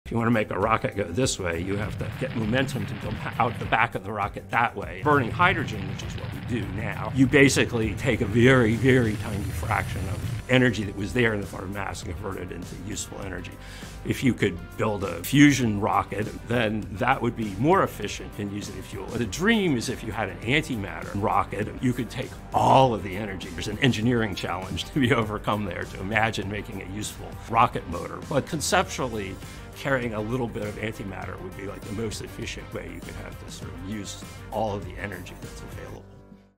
Antimatter Rockets?
What is antimatter? What can it tell us about the universe? We asked a physicist! Dr. Kevin Jones, a Program Director with the National Science Foundation's Atomic, Molecular and Optical Physics program, explained what antimatter is and why this field of study could hold secrets beyond our current understanding.